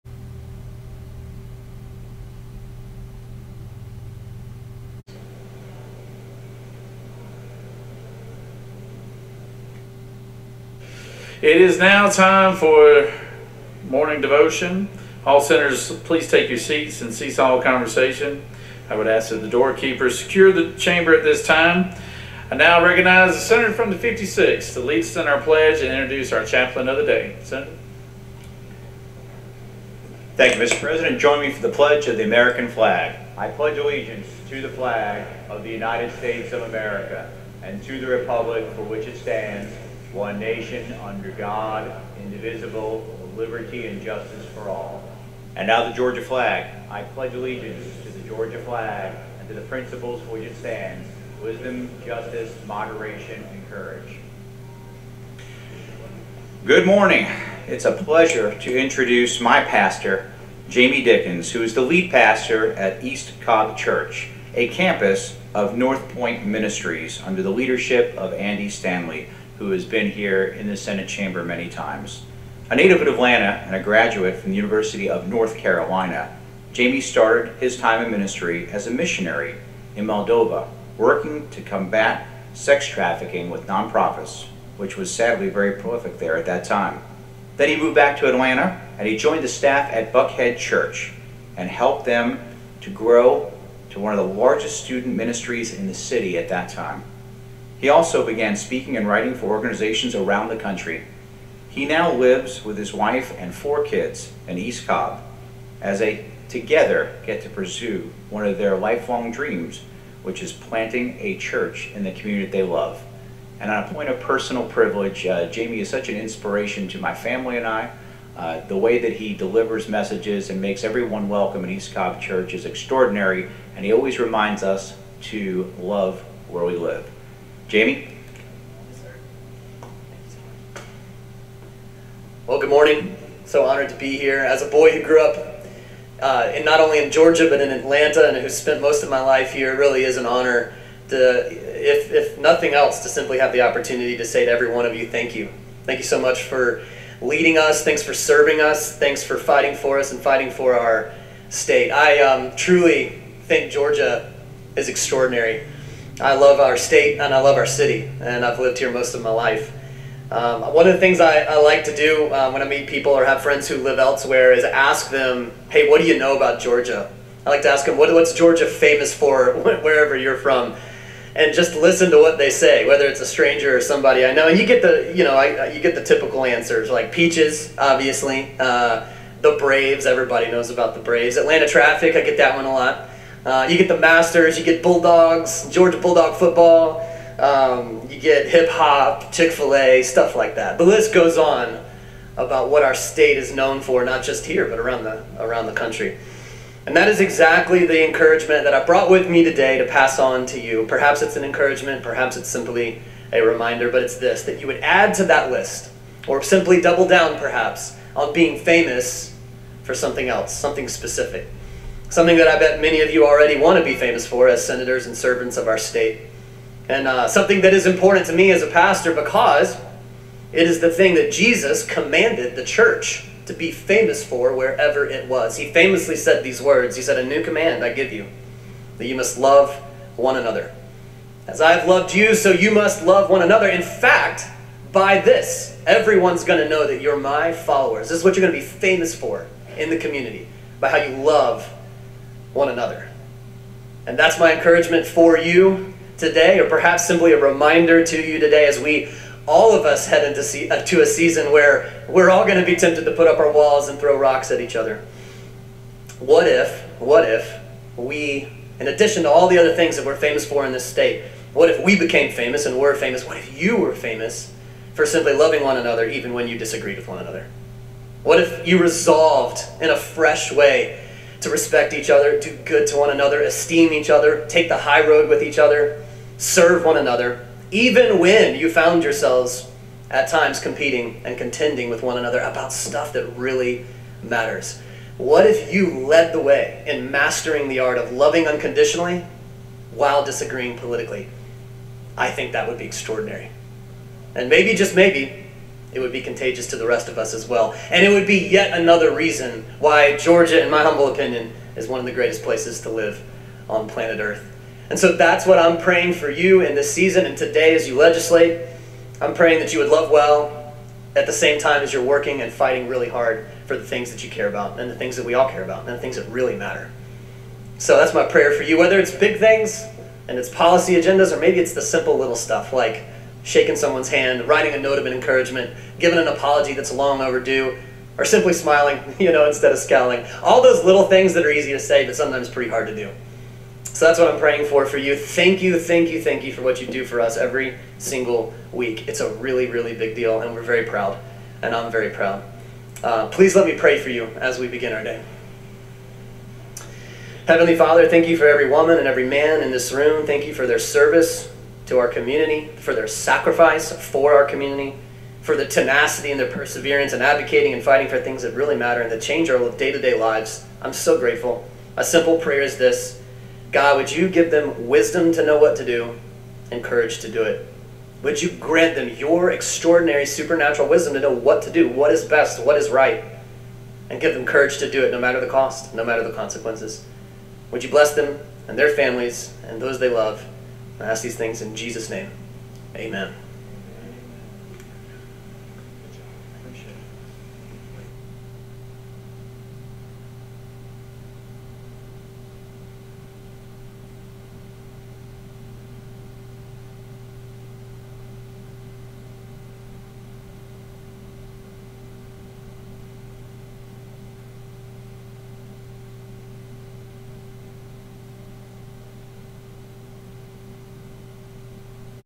Lead Pastor Jamey Dickens of East Cobb Church in Marietta, Georgia — GA Senator John Albers 03/14/24
I pledge allegiance to the flag of the United States of America and to the Republic for which it stands, one nation under God, indivisible, with liberty and justice for all.

I pledge allegiance to the Georgia flag and to the principles for which it stands: Wisdom, Justice, Moderation and Courage.

SENATOR JOHN ALBERS: Good morning. It's a pleasure to introduce my Pastor Jamey Dickens at East Cobb Church.

Well, good morning. So honored to be here. As a boy who grew up in — not only in Georgia, but in Atlanta and have spent most of my life here, it really is an honor to — if nothing else to simply have the opportunity to at least to say to every one of you: Thank you. Thank you so much for leading us, thanks for serving us, thanks for fighting for us and fighting for our state.

I truly think Georgia is extraordinary. I love our state and I love our city and I've lived here most of my life. One of the things I like to do when I meet people or have friends who live elsewhere is ask them: Hey, what do you know about Georgia? I like to ask them: What's Georgia famous for wherever you're from? and just listen to what they say whether it's a stranger or somebody I know. You get, you know, you get the typical answers like: Peaches, obviously; the Braves, everybody knows about the Braves; Atlanta traffic, I get that one a lot; you get the Masters; you get the Bulldogs — Georgia Bulldogs football; you get hip hop; Chick-fil-A, stuff like that; the list goes on about what our state is known for, not just here, but around the country.

And that is exactly the encouragement that I brought with me today to pass on to you. Perhaps, it's an encouragement. Perhaps, it's simply a reminder, but it's this: that you would add to that list or simply double-down, perhaps on being famous for something else — something specific. Something that I bet many of you already want to be famous for as Senators and as servants of our state and something that is important to me as a pastor because it is the thing that Jesus commanded the church to be famous for wherever it was. He famously said these words, He said: A new command I give you that you must love one another as I have loved you. So, you must love one another, in fact, by this everyone's gonna know that you're my followers. This is what you are going to be famous for in the community about how you love one another.

And that's my encouragement for you today and perhaps simply a reminder to you today as we — all of us head into a season where we're all going to be tempted to put up our walls and throw rocks at each other.

What if we — in addition to all the other things we are famous for in this state — what if we became famous for simply loving one another even when you disagreed with one another. What if you resolved in a fresh way to respect each other, do good to one another, to esteem each other; take the high road with each other; serve one another even when you found yourselves at times competing and contending with one another about stuff that really matters. What if you led the way in mastering the art of loving unconditionally while disagreeing politically? I think that would be extraordinary and maybe just maybe it would be contagious to the rest of us as well and it would be yet another reason why Georgia in my humble opinion is one of the greatest places to live on planet Earth.

And so that's what I'm praying for you in this season and today as you legislate. I'm praying that you would love well at the same time as you are working and fighting really hard for the things that you care about and things that we all care about and things that really matter. So that's my prayer for you whether it's big things and policy agendas or maybe it's the simple little stuff like shaking someone's hand, writing a note of encouragement, giving an apology that's long overdue or simply smiling instead of scowling. All those little things that are easy to say but are sometimes pretty hard to do. So that's what I'm praying for for you. Thank you for what you do for us every single week. It's a really, really big deal and we're very proud and I'm very proud. Please let me pray for you as we begin our day.

Heavenly Father, thank you for every woman and every man in this room. Thank you for their service to our community, for their sacrifice for our community; for their tenacity and their perseverance in advocating and fighting for things that really matter and that change our day-to-day lives. I'm so grateful. A simple prayer is this: God, would you give them wisdom to know what to do and courage to do it. would you grant them your extraordinary supernatural wisdom to know when what to do what is right and give them courage to do it no matter the cost, no matter the consequences. Would you bless them and their families and those they love. I ask these things in Jesus name. Amen.